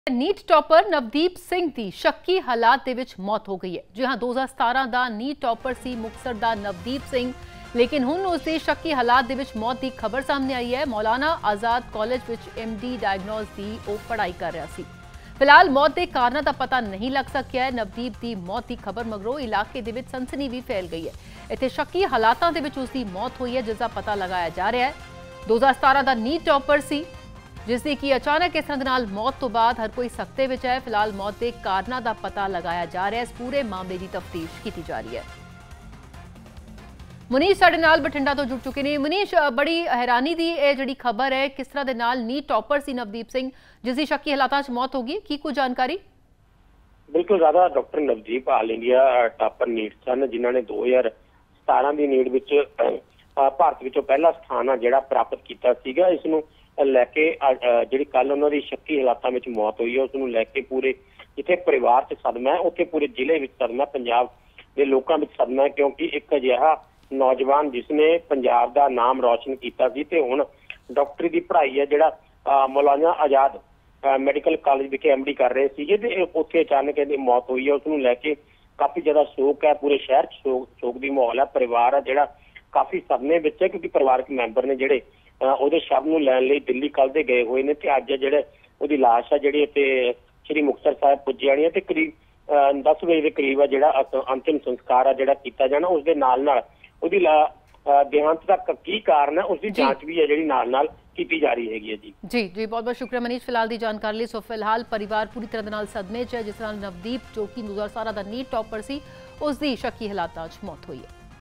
NEET 'ਚ ਆਲ ਇੰਡੀਆ ਟਾਪਰ ਰਹੇ ਵਿਦਿਆਰਥੀ Navdeep Singh ਦੀ ਸ਼ੱਕੀ ਹਾਲਾਤਾਂ 'ਚ ਮੌ+ਤ | NEET Topper Navdeep Singh
NEET 'ਚ ਆਲ ਇੰਡੀਆ ਟਾਪਰ ਰਹੇ ਵਿਦਿਆਰਥੀ  Navdeep Singh  ਦੀ ਸ਼ੱਕੀ ਹਾਲਾਤਾਂ 'ਚ ਮੌ+ਤ, ਨਵਦੀਪ ਸਿੰਘ ਦੀ ਮੌ+ਤ ਮਗਰੋਂ ਸਦਮੇ 'ਚ ਪਰਿਵਾਰ

Navdeep Singh
 NEET Topper Navdeep